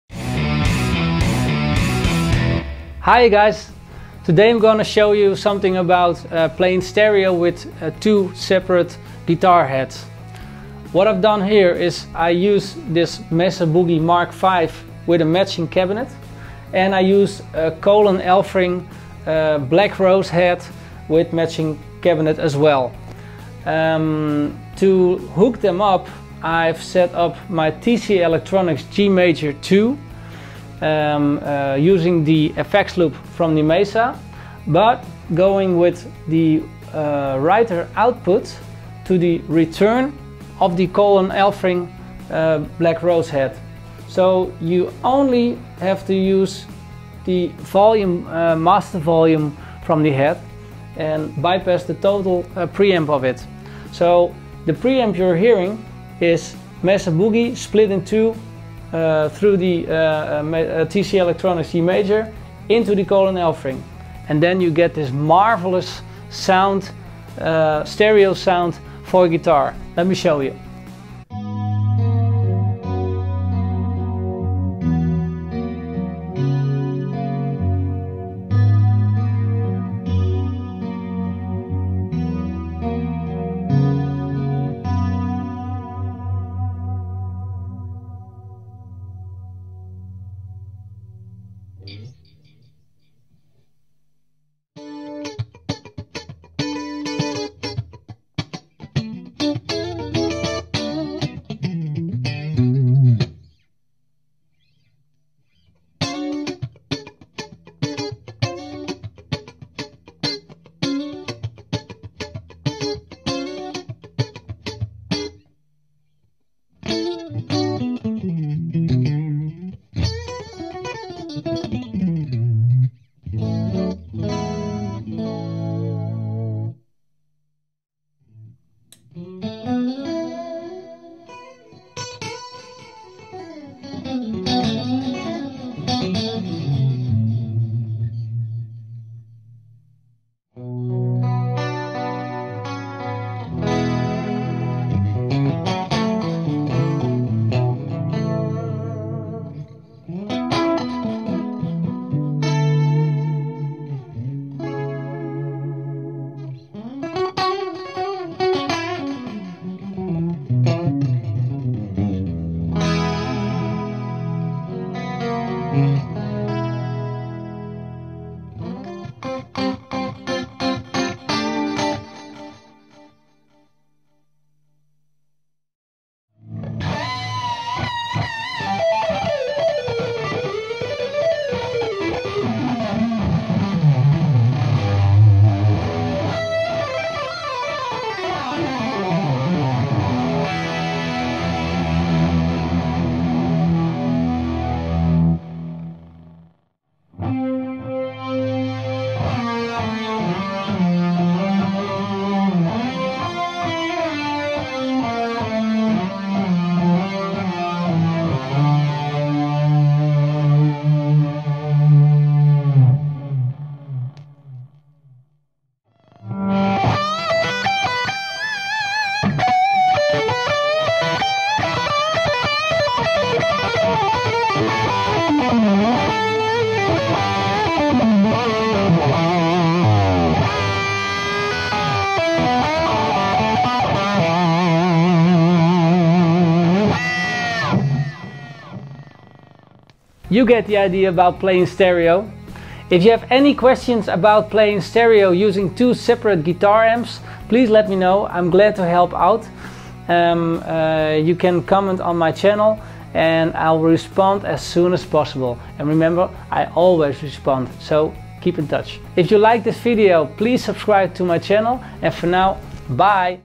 How to sound like Steve Lukather (80s-90s) | Mesa Mark V in Stereo setup | Musicman Luke II BFR
In this video I'm gonna show you How To Create Stereo Guitarsound Using Two Separate Amps using the FX loops of both amps.

I'm using a Mesa Mark V + 2x12 cab, a Kool & Elfring Black Rose + 2x12 cab and a TC Electronics Gmajor2.
The guitar I'm using is a MusicMan Luke 2 BFR Tobacco Quilted with gold hardware.

Sounds:
1:11 - 1:30 Clean - Deep Chorus + Dual Delay
1:37 - 1:44 Clean - Chorus + Compressor
1:45 - 2:07 Clean - Chorus + Compressor
2:09 - 2:52 Clean - Deep Chorus + Detuner + Dual Delay
2:57 - 3:15 High Gain - Chorus + PreDelay + Dual Delay
3:17 - 3:33 High Gain - Chorus + Detune + PreDelay + Dual Delay (Luke!)
3:37 - 3:56 High Gain - Dual Delay